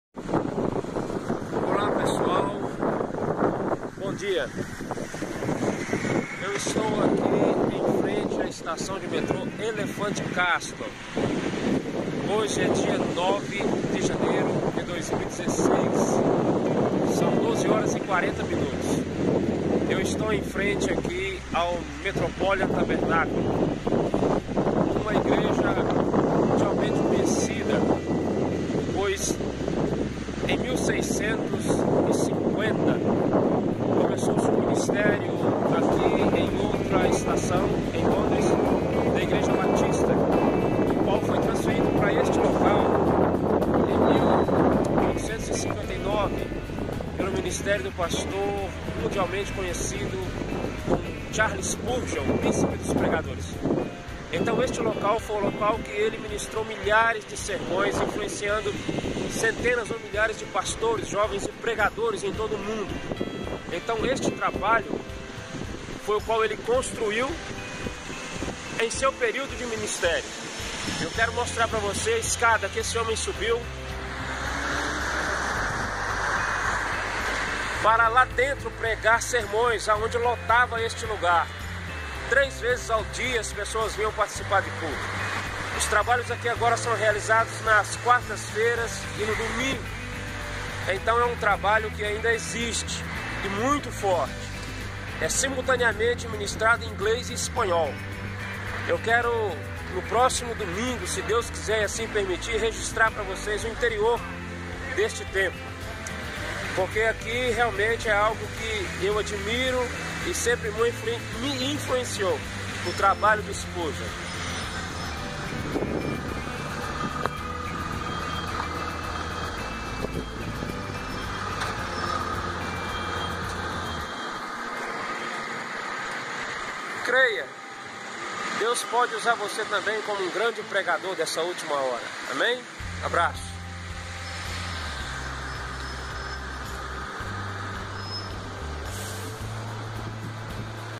Igreja de Spurgeon em Londres
Igreja construída em Londres no séc XVIII por Charles Spurgeon